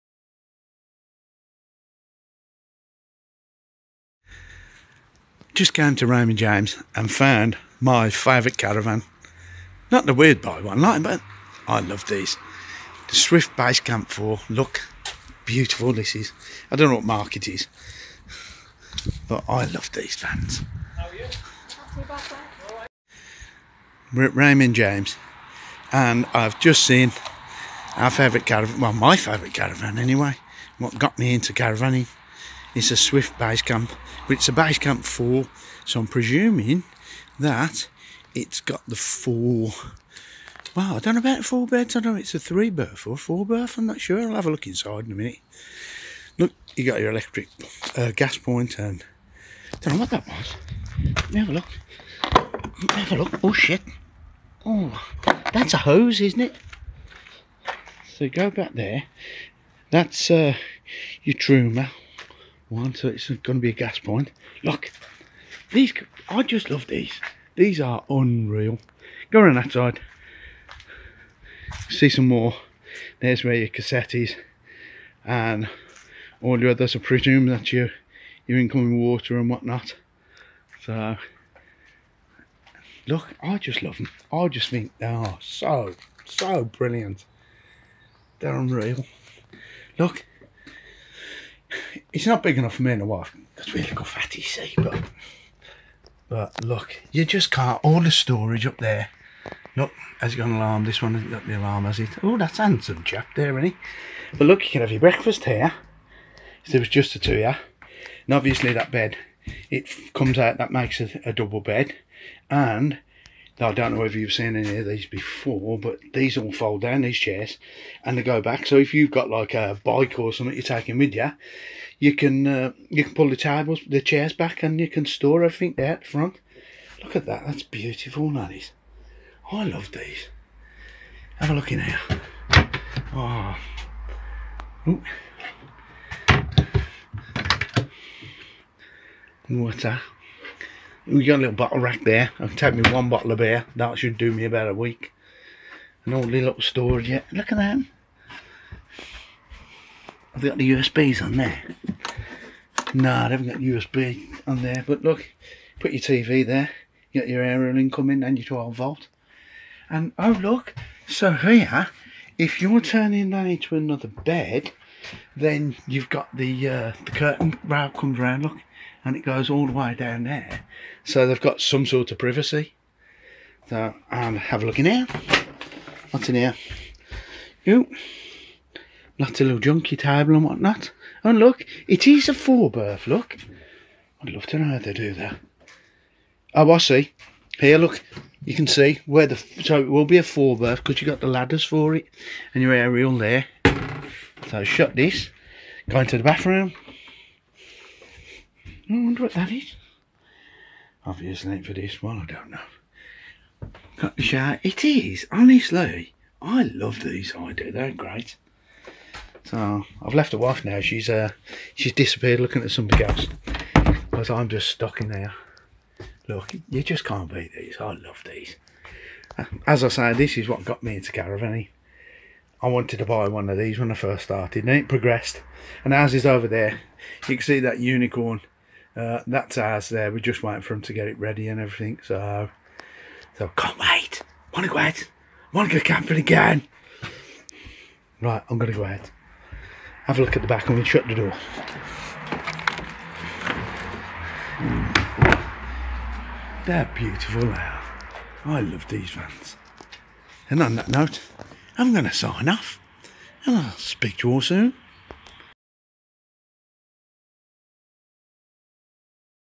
Swift Basecamp 4
This is my favourite caravan, it's what drew me into Caravaning in the beginning ☺️